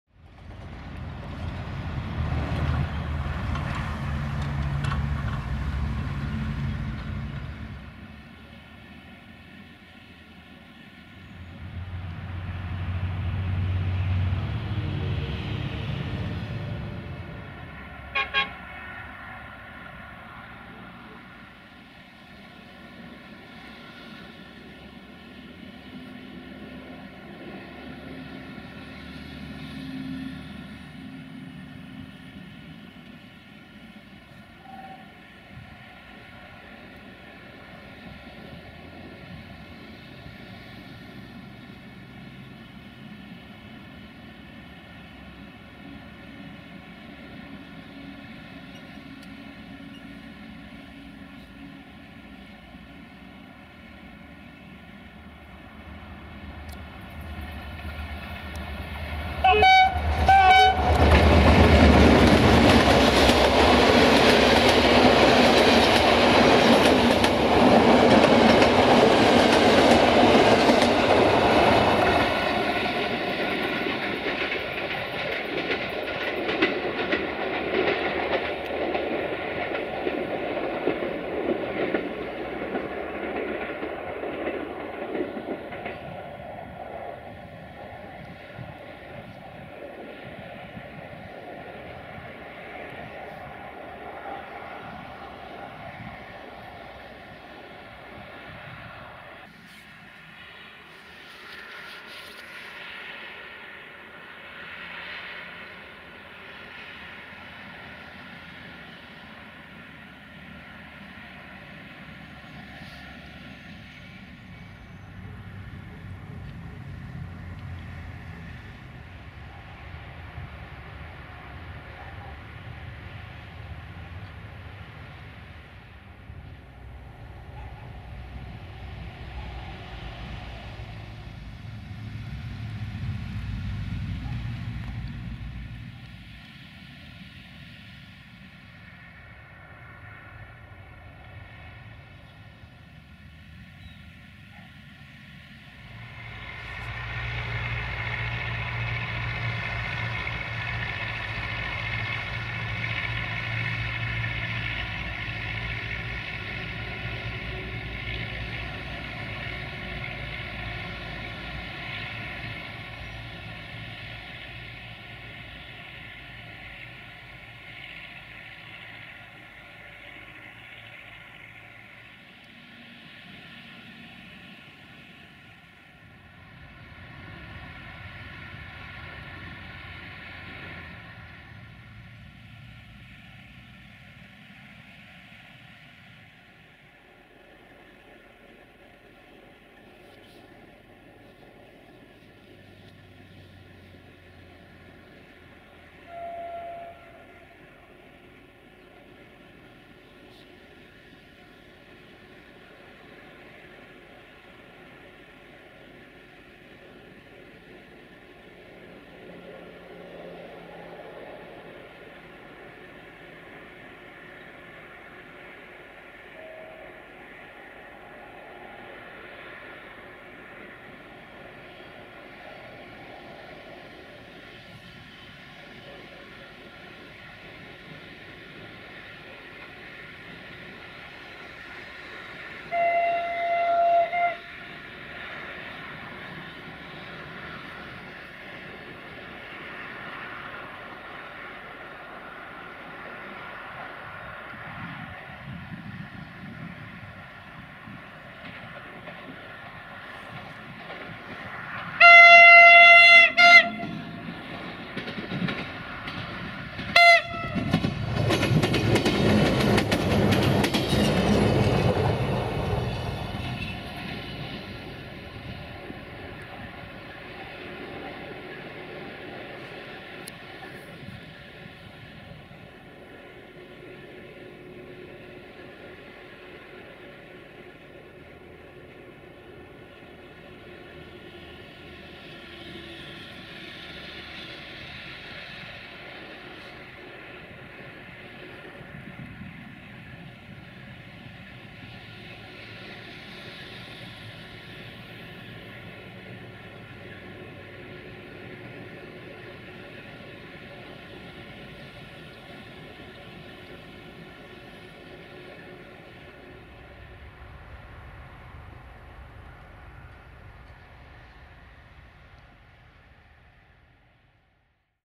Două Trenuri de Călători/Two Passenger Trains in Gara Oradea Vest Station - 09 April 2021
00:22 IR1833 - Iași-Târgu Frumos-Pașcani-Dolhasca-Verești-Suceava-Gura Humorului-Frasin-Vama-Câmpulung Est-Câmpulung Moldovenesc-Pojorâta-Mestecăniș-Iacobeni-Vatra Dornei-Vatra Dornei Băi-Lunca Ilvei-Măgura Ilvei-Poiana Ilvei-Ilva Mică-Năsăud-Salva-Beclean pe Someș-Dej Călători-Gherla-Cluj Napoca-Aghireș-Huedin-Ciucea-Bratca-Șuncuiuș-Aleșd-Oradea-Salonta-Ciumeghiu-Chișineu Criș-Sântana-Arad-Timișoara Nord - EGM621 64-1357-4  RO-SNTFC
02:27 Tractor U650 Tractor
03:08 IR1733 - Timișoara Nord-Aradu Nou-Arad-Sântana-Chișineu Criș-Ciumeghiu-Salonta-Oradea-Săcuieni Bihor-Valea lui Mihai-Carei-Satu Mare-Odorheu-Medieșu Aurit-Apa-Seini-Baia Mare - Automotor Săgeată Albastră Desiro VT642 DMU RO-SNTFC

09 April 2021